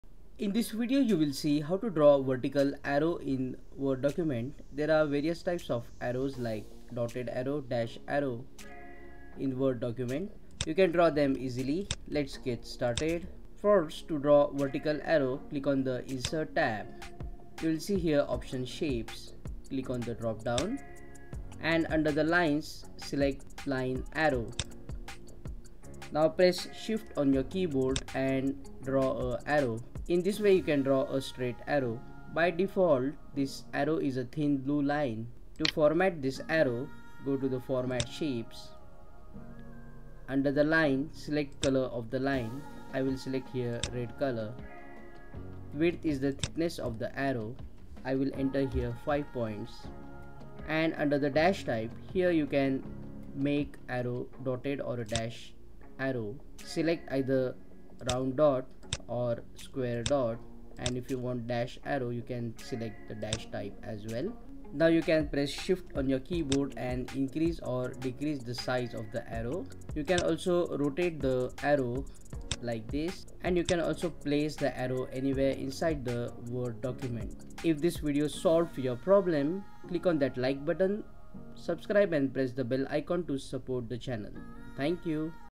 How to Draw Vertical Arrow in Word
How to Draw Vertical Arrow in Word dotted or dashed is shown in this video. To make vertical arrow straight, press shift on the keyboard and then draw the vertical line arrow